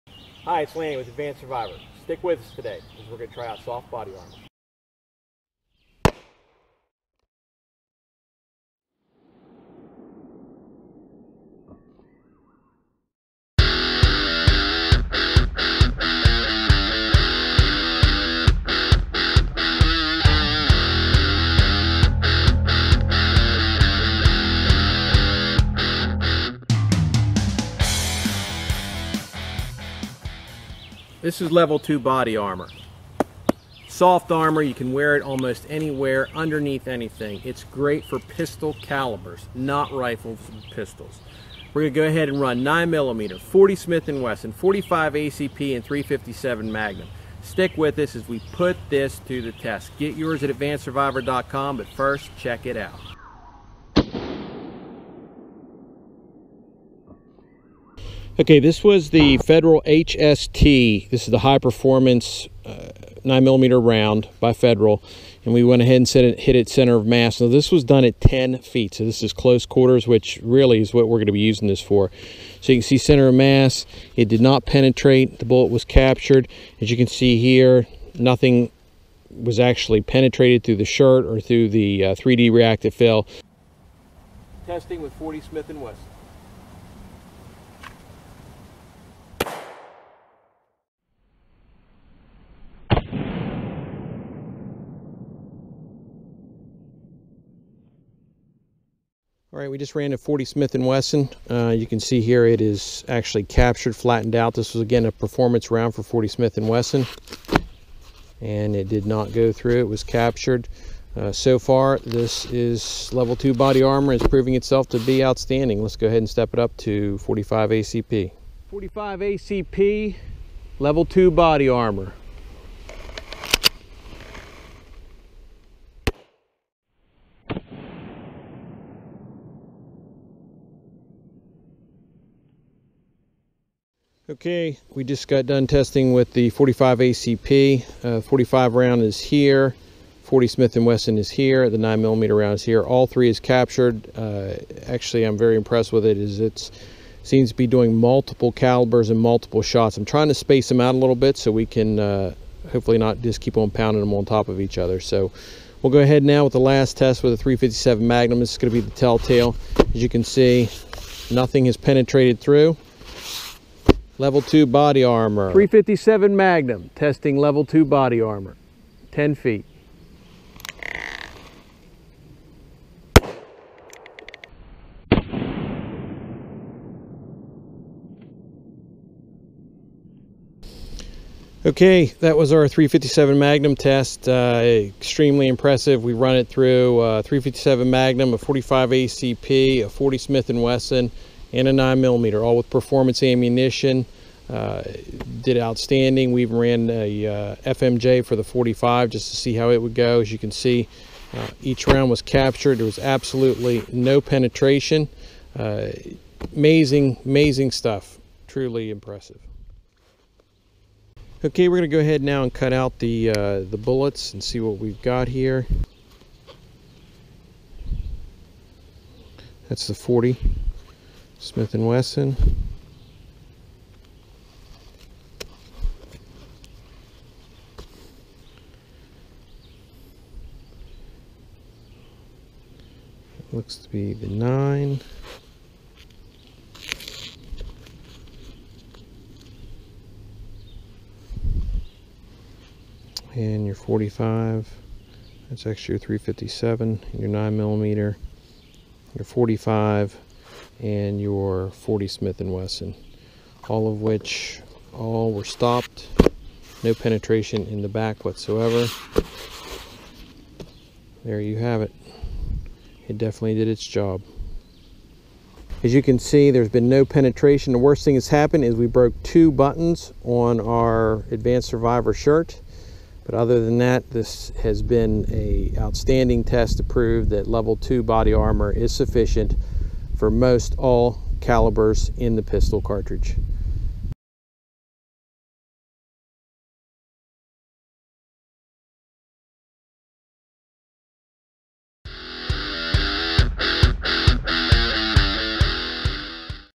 Advanced Survivor movie soft armor HD
Advanced Survivor Testing Level 2 Body Armor with various Pistol Cartridges.